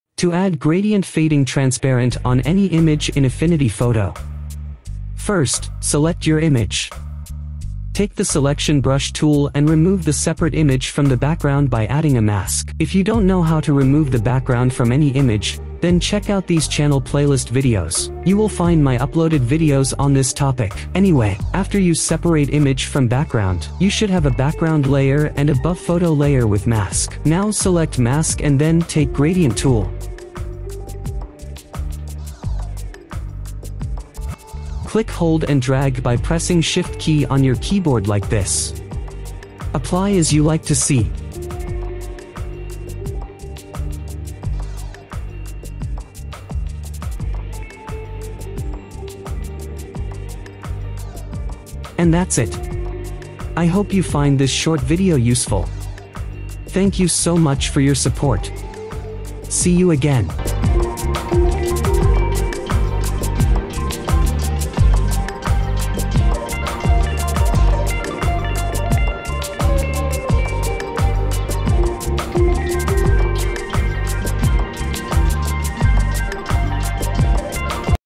How To Add Gradient Fading Transparent on any image In Affinity Photo
Hello everyone,
I'm Dav, and in this video, you’ll know exactly how to add Gradient Fading transparency to any image in Affinity Photo. This is an Affinity Photo Tutorial on adding gradient fading transparency to any image. How to Create Image Gradient Transparency Fade Effect in Affinity Photo

👉 Please watch his video till the end. I appreciate your LIKE / COMMENT/ SHARE !! 🙂